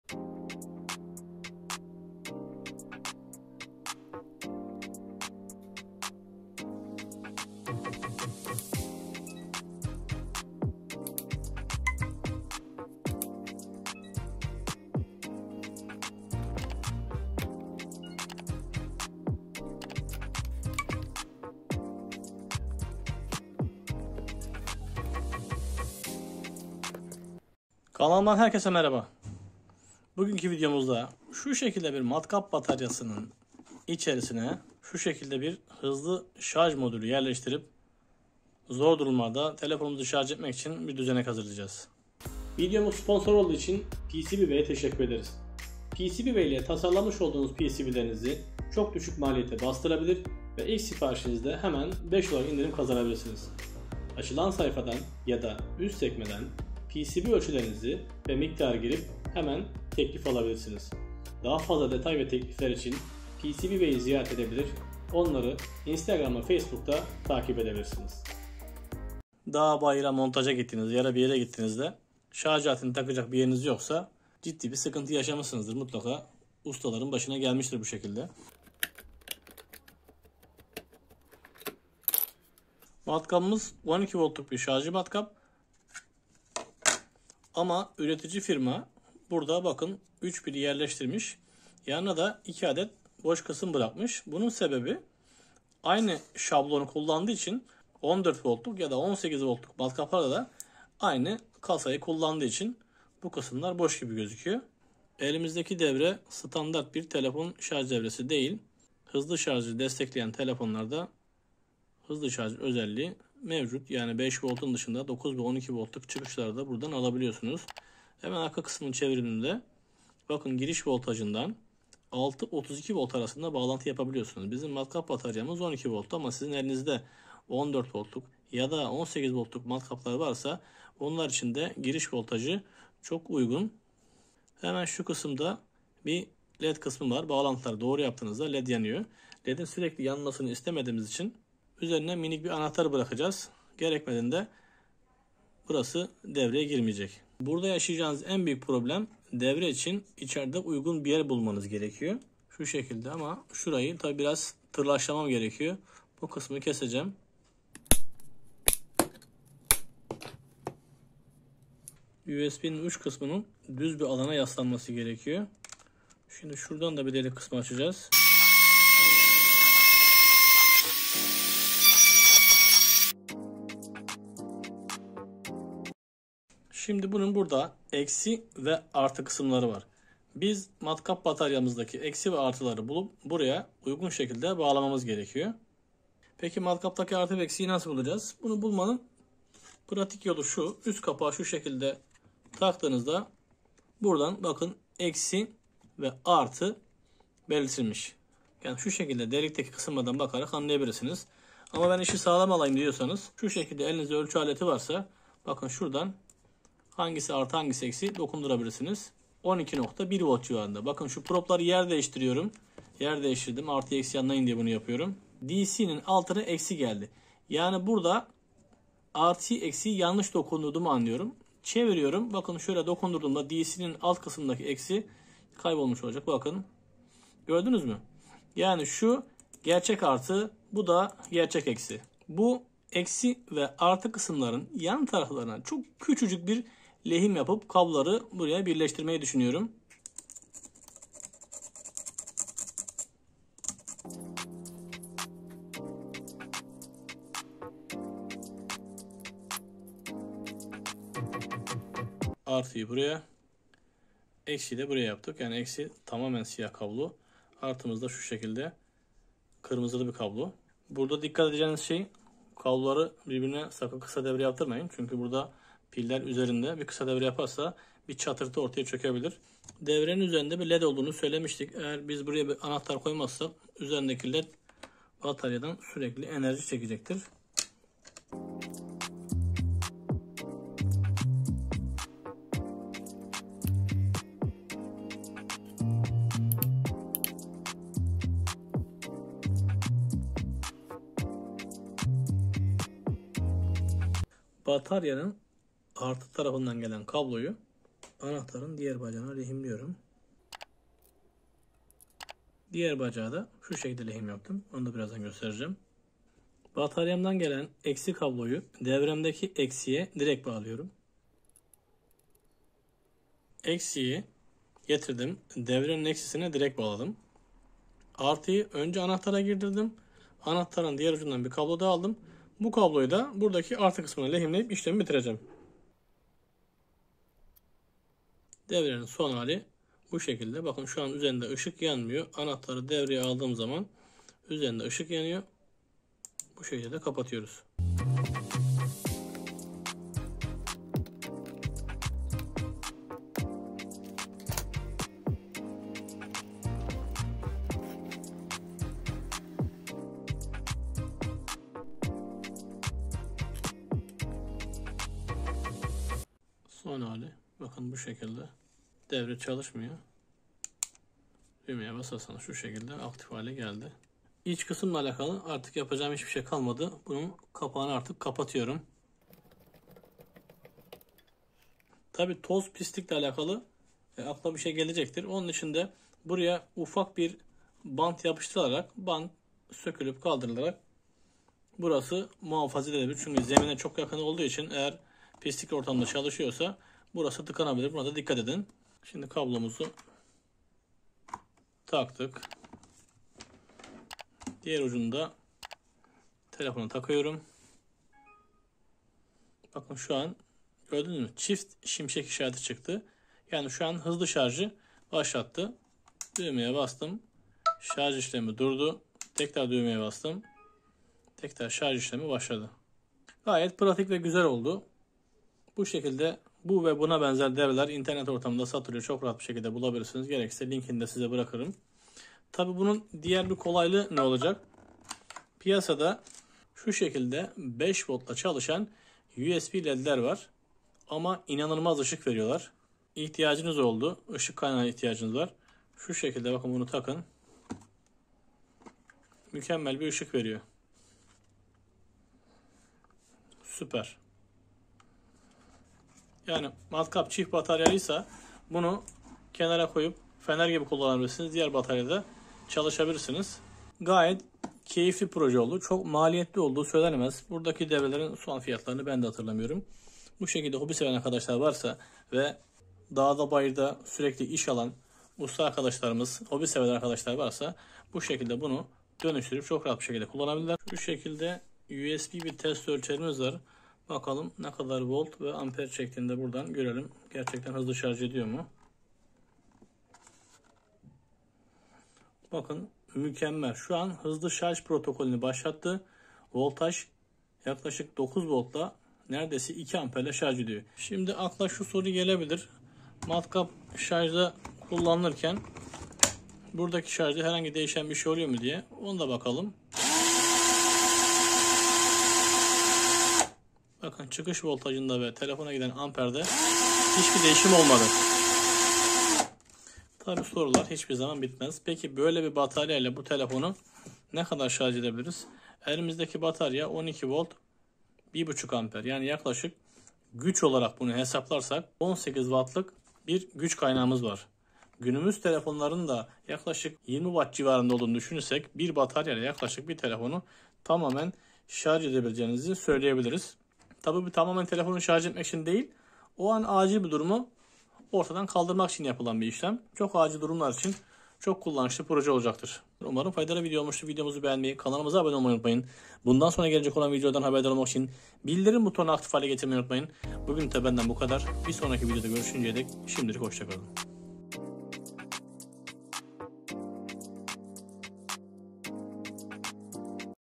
Akülü Matkap İle Telefon Şarj Edin! Matkap Bataryası İle Telefon Şarjı, Şaşırtıcı Bir Telefon Şarjı
Bu videomuzda Akülü Matkap İle Telefon Şarj Edin! Matkap Bataryası İle Telefon Şarjı, Şaşırtıcı Bir Telefon Şarjı videosu yaptık..